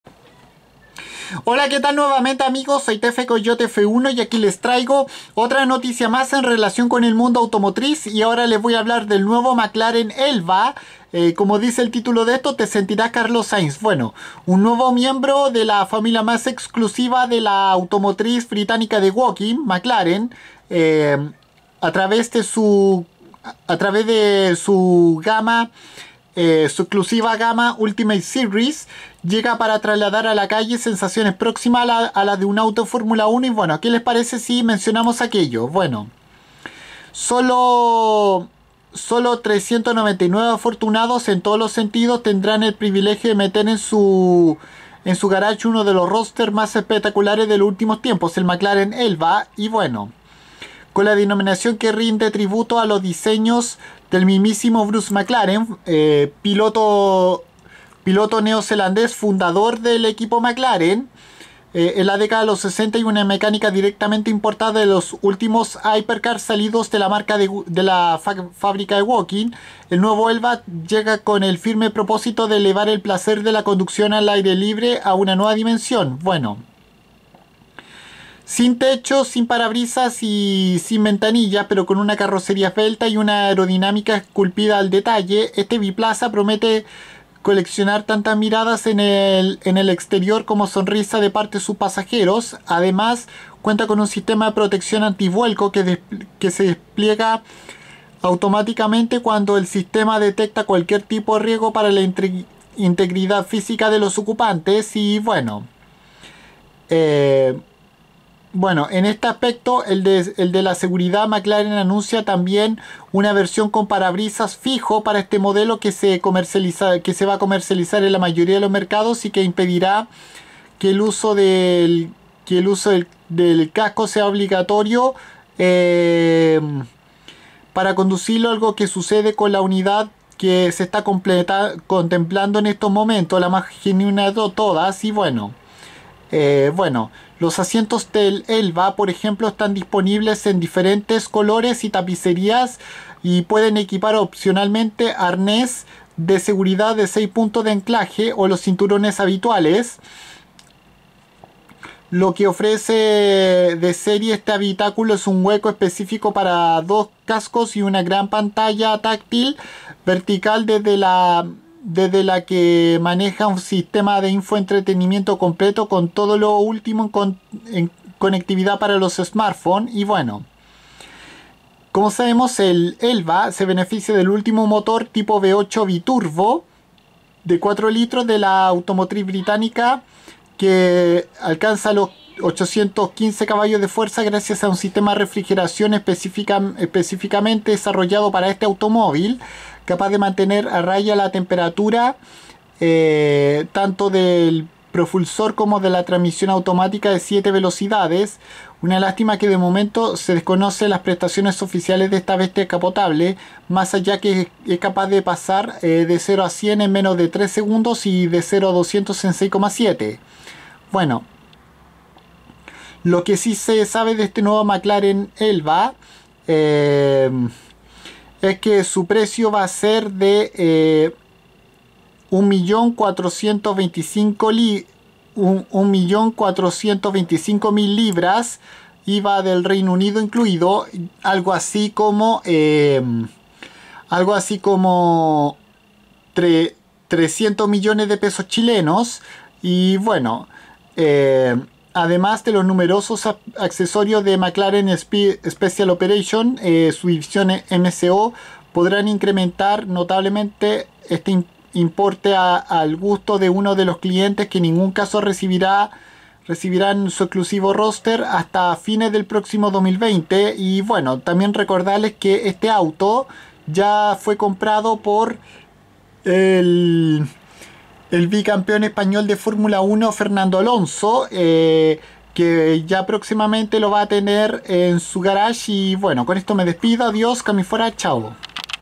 Mclaren Elva información
Aquí les hablo de este exclusivo superdeportivo de la marca británica de Woking del cual solo habrán 399 unidades y que esta inspirado en los Fórmula 1

También cabe destacar que el asturiano Fernando Alonso ya compró uno de estos modelos

No llegarán a producirse hasta finales de 2020